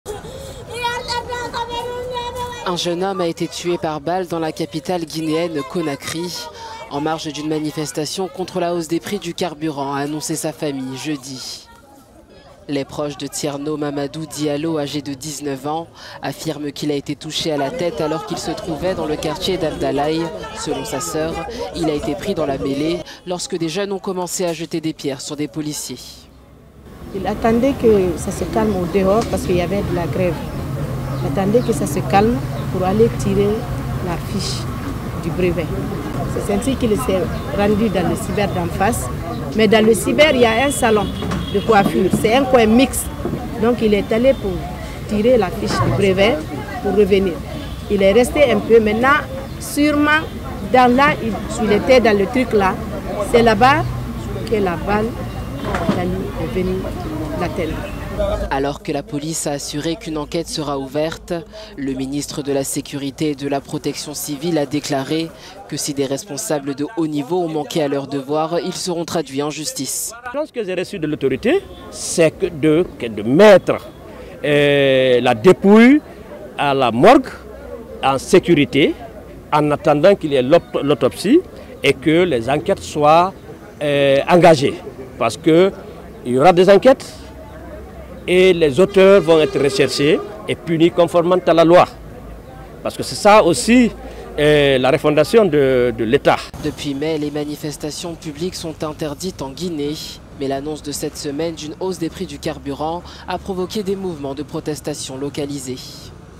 Guinée : un mort par balle lors d'une manifestation
Un jeune homme a été tué par balle à Conakry en marge d'une manifestation contre la hausse des prix du carburant, a annoncé jeudi sa famille.